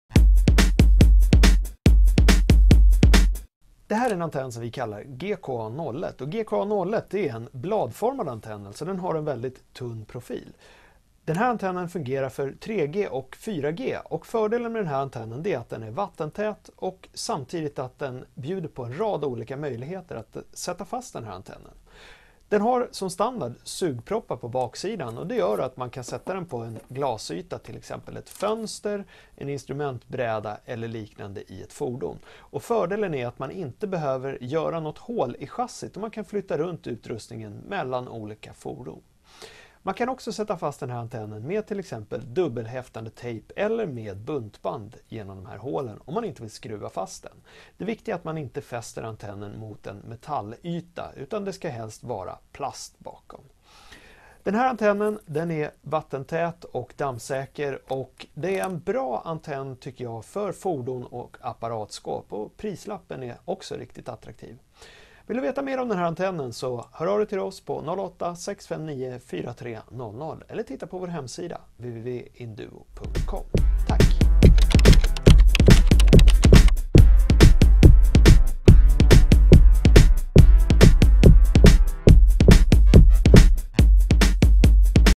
GKA01 Vattenskyddad och kompakt 4G-antenn med MIMO
GKA01 ger snabb och enkel montering i fönster, på instrumentbräda eller apparatskåp. Antennen levereras med 2 x 1 m-kablar och SMA kontakter.